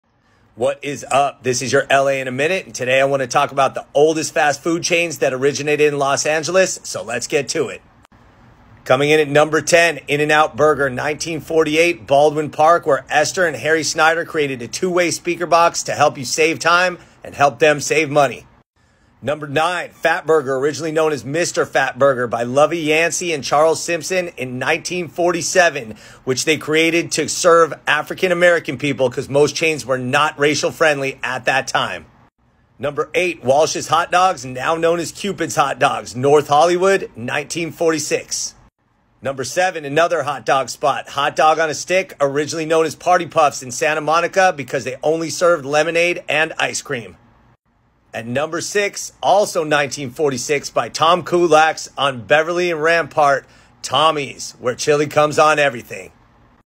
Oldest fast food chains in Los Angeles, part I
Erewhon is known as an influencer haven; With $22 smoothies, collagen water, and more probiotic and kombucha then you can dream of, it is the consummate Los Angeles grocery store. Here are five things you didn’t know about the L.A.-based chain.
This is your L.A. in a Minute.